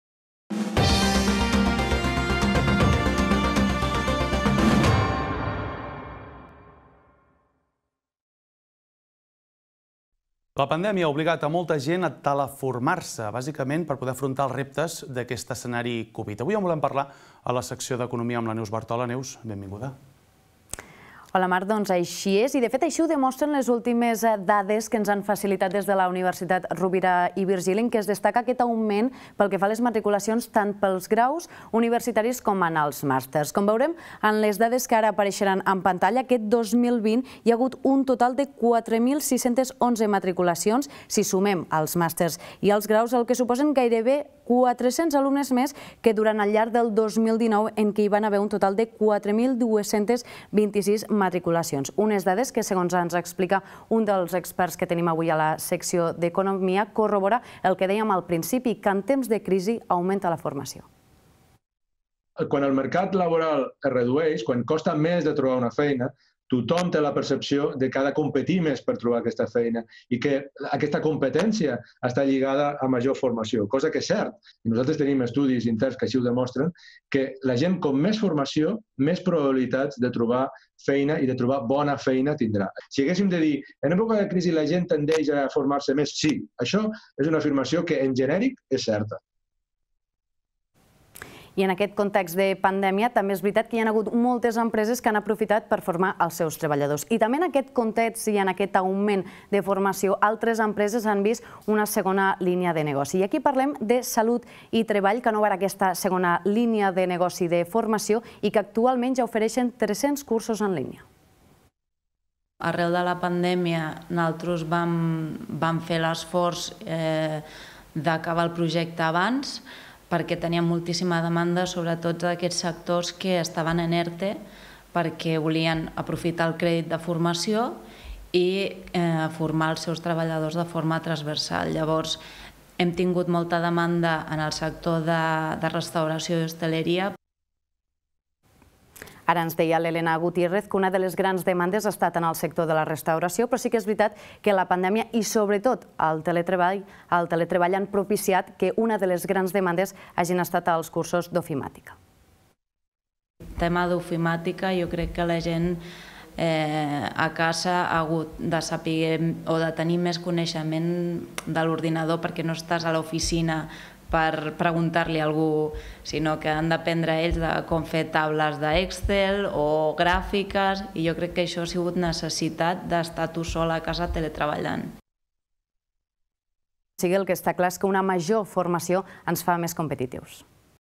ECONOMIA | La demanda de formació augmenta en temps de crisi
Les matriculacions en Graus i Màsters de la Universitat Rovira i Virgili han augmentat en un 9% per cent. Una dada que confirma la teoria que en època de crisi, augmenta la formació. Per aquest motiu, i davant l'augment de demanda, hi han hagut empreses que han trobat, en aquesta formació, una nova línia de negoci.